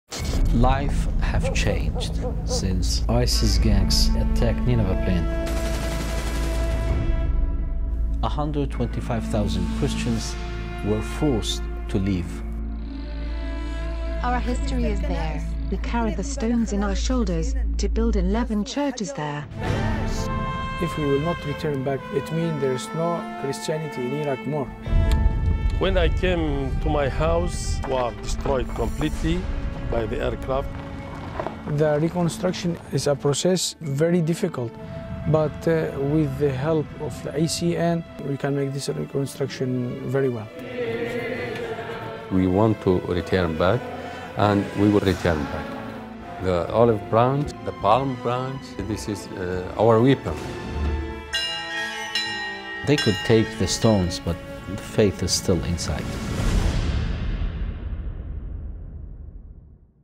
Ajude os Cristãos a sobreviver na Planície de Nínive | Iraque
A vida mudou desde que ISIS atacou a Planície de Nínive
125.000 Cristãos foram forçados a abandonar a sua terra

O Cristianismo no Iraque está prestes a desaparecer.

A reconstrução é um processo muito difícil, mas contam com o apoio da Fundação AIS.

Os Cristãos do Iraque querem voltar às suas casa. Os ramos de oliveira e palmeira serão as suas armas.

Ajude os Cristãos a sobreviver na Planície de Nínive!